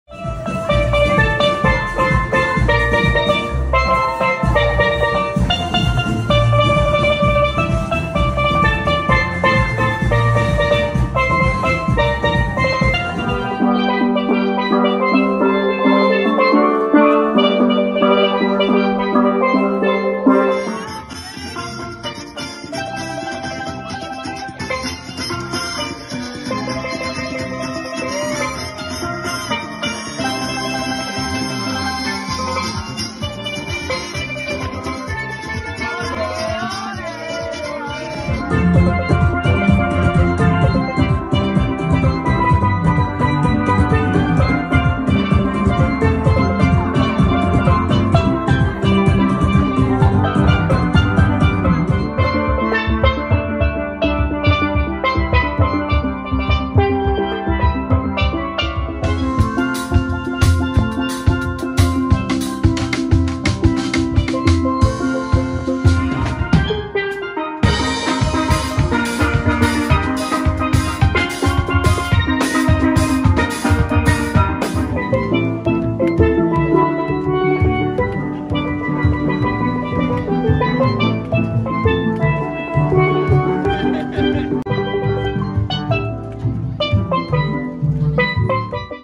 Melodic Island Sounds Showreel - AliveNetwork.com
From the impressive grounds of Lord's Cricket Ground to the high-speed thrills of Silverstone Grand Prix, Melodic Island Sounds are a professional steel pan collective who have entertained for esteemed clients such as British Airways, John Lewis, YouTube, Sky, Warner Bros and many more! Tailoring their performances to suit, the collective can perform as a soloist to full 6 piece band!